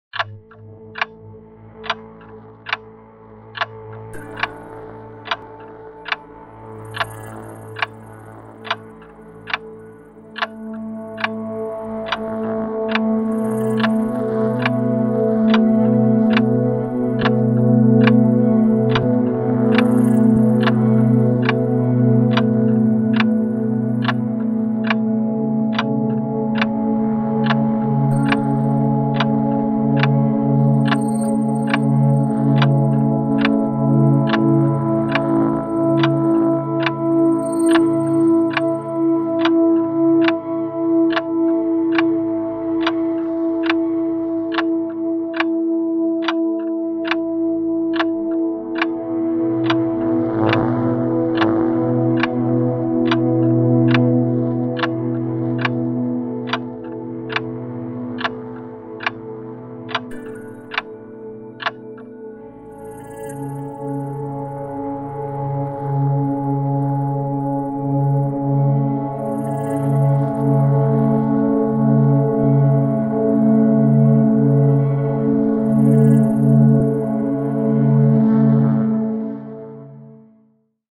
Tick Tock
The clock is ticking on your chance to register for Delta Chi's 59th International Convention in Scottsdale, on July 30th - August 3rd. before time... runs out.